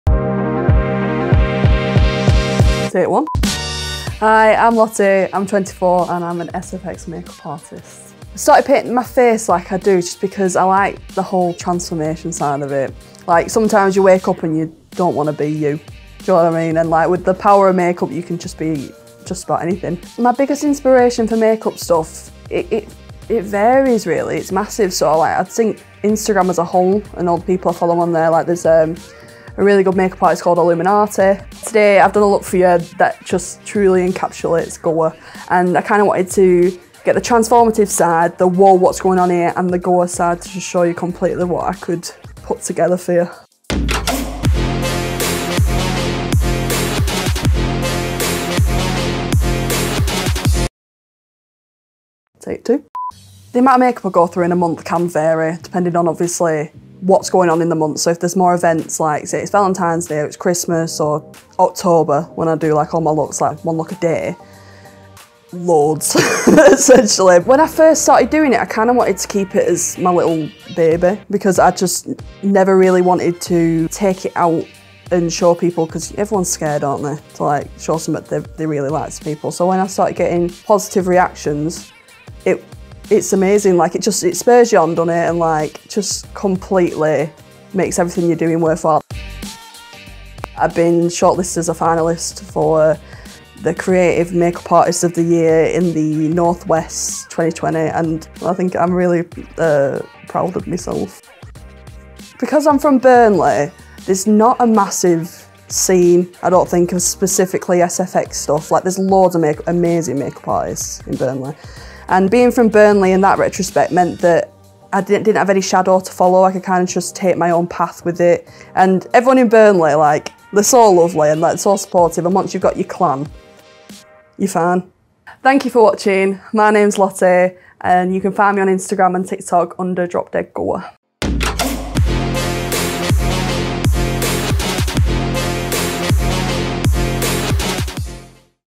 Burnley Uncovered | Episode 16 | Show us your brains - Lottie May Taylor
Special effects makeup continues to blow our minds when we watch TV shows like The Witcher, Game of Thrones and even RuPaul's Drag Race (have you seen some of the looks they serve!?). It comes as no surprise that we were immediately obsessed when we first saw Lottie May Taylor's INCREDIBLE makeup skills.

We took over Illuminati to show you exactly what this SFX makeup artist can do.

Search on TikTok: @dropdeadg0re